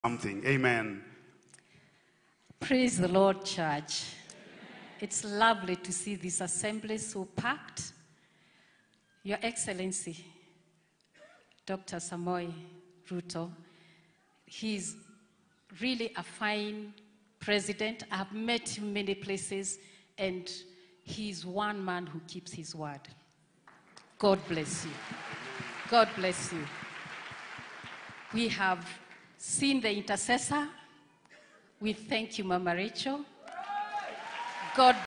RUTO IS A FINE MAN!! SEE HOW THIS WOMAN DESCRIBES PRESIDENT RUTO IN CHURCH
PRESIDENT RUTO IN CHURCH